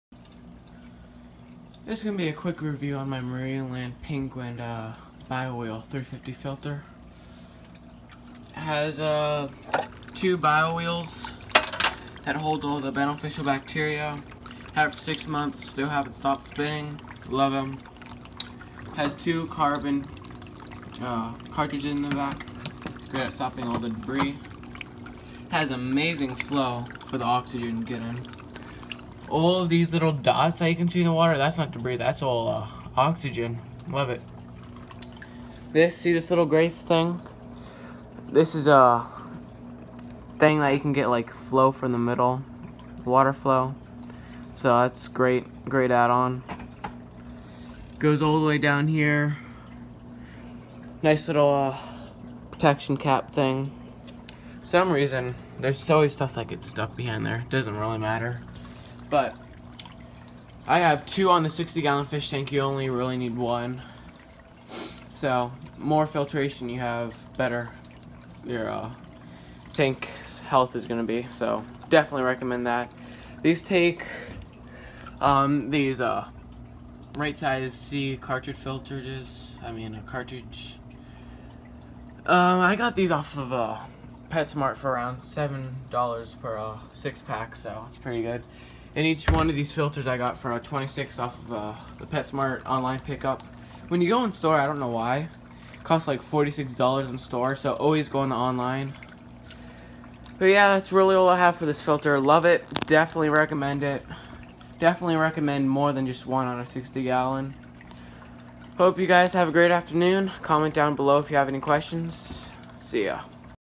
penguin 350 bio wheel power filter review
Love this filter!, got it off of PetSmart online pickup for $26. penguin 350 filter takes rite Size C filter cartridge. 60 gallon tank, hope this helped with your filter choice!